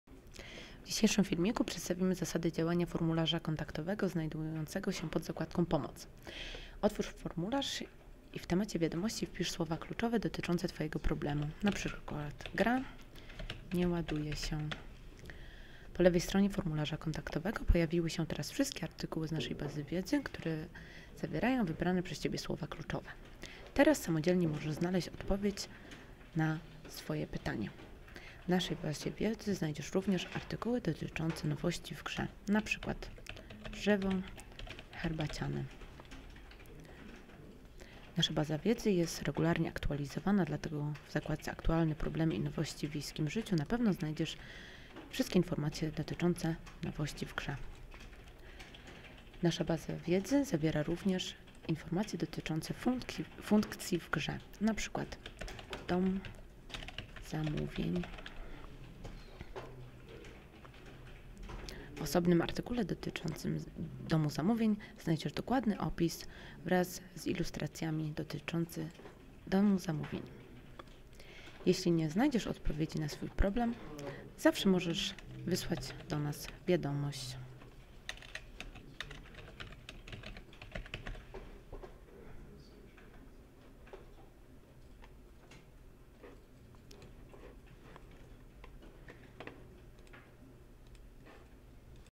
formularz kontaktowy
Otwórz formularz kontaktowy poprzez kliknięcie na zakładkę "Pomoc", znajdującą się tuż nad oknem gry
Wpisz w pierwszym okienku słowo kluczowe dotyczące Twojego problemu
Po lewej stronie formularza kontaktowego pojawiły się teraz wszystkie artykuły z naszej Bazy Wiedzy, które zawierają wybrane przez Ciebie słowo kluczowe
Teraz samodzielnie możesz szybko znaleźć rozwiązanie problemu
"W naszej bazie wiedzy znajdziesz również artykuły dotyczące nowości w grze.
W naszej bazie danych znajdziesz również osobne arytkuły dotyczące funkcji w grze.
Nasza Baza Wiedzy jest ciągle aktualizowana. Sprawdź więc przed wysłaniem wiadomości do nas, czy Twój problem nie został już w niej opisany."
Jeżeli nie znajdziesz odpowiedzi na swój problem, możesz oczywiście zawsze wysłać do nas zgłoszenie